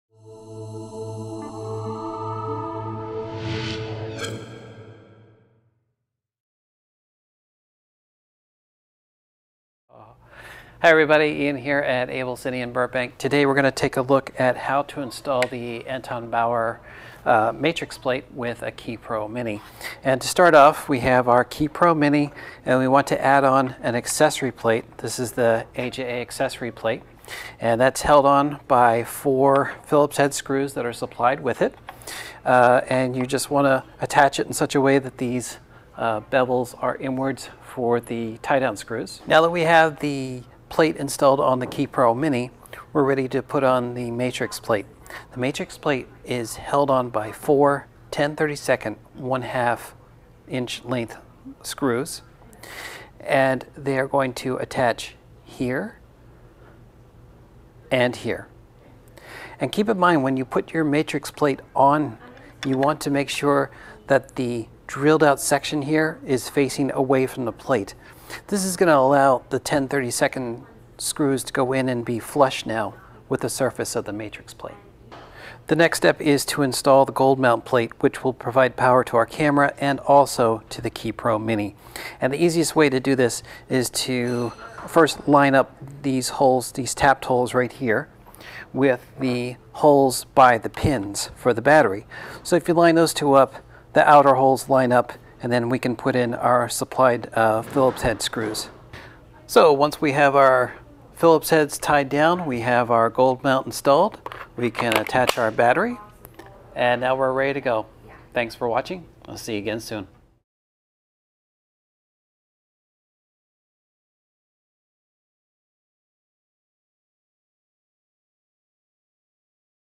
At the Bench with the Anton/Bauer Matrix Plate
I'm often asked about the available solutions for attaching accessories or external recorders to camera rigs. One solution for 15mm lightweight or 19mm rods is the new Anton/Bauer MATRIX cheese plate. This versatile cheese plate allows for both vertical and horizontal mounting, and it fits most standard accessories. Watch my video above as I detail how to attach an AJA KiPro Mini to the MATRIX plate.

For more info on the Anton/Bauer MATRIX plate, visit our product